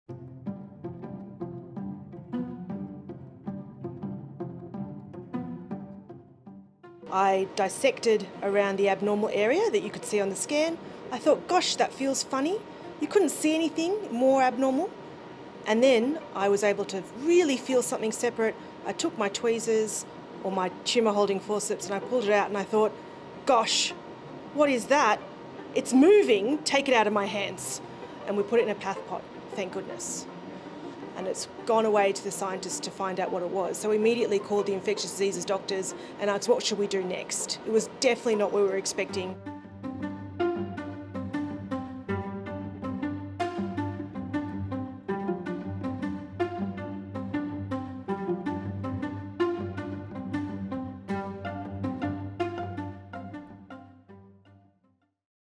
Roundworm pulled from woman's brain leaves doctors stunned
A neurosurgeon performing a biopsy pulled a roundworm, known as a parasite that infects snakes, from a woman's brain.

In what scientists are calling a world first, a 3-inch worm was found alive after doctors performed surgery on the brain of an Australian woman.

The worm was pulled from a 64-year-old woman last year, who had been suffering from symptoms including stomach pain, cough, forgetfulness and depression, according to the findings documented in a study in the Emerging Infectious Diseases Journal from the Centers for Disease Control and Prevention.